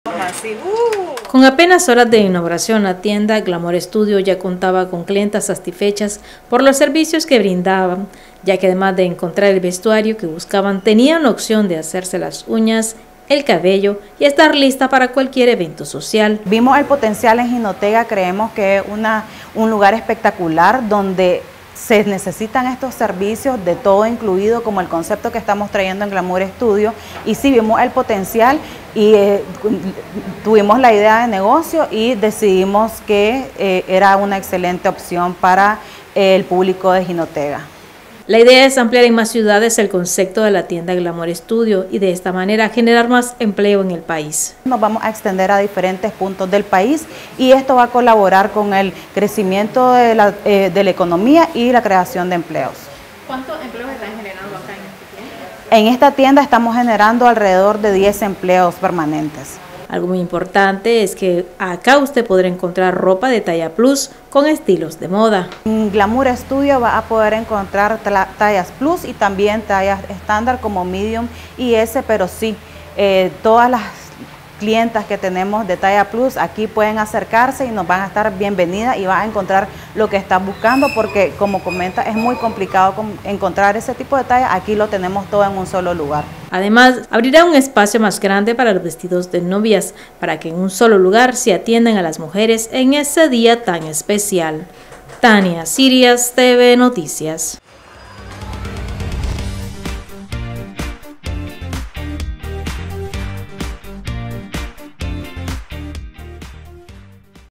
Glamour Studio abre sus puertas en Jinotega con un nuevo concepto de atención
Un nuevo concepto de tienda abrió sus puertas en Jinotega, se trata de Glamour Studio, donde además de comprar ropa y accesorios, las féminas pueden hacerse las uñas o arreglarse el cabello.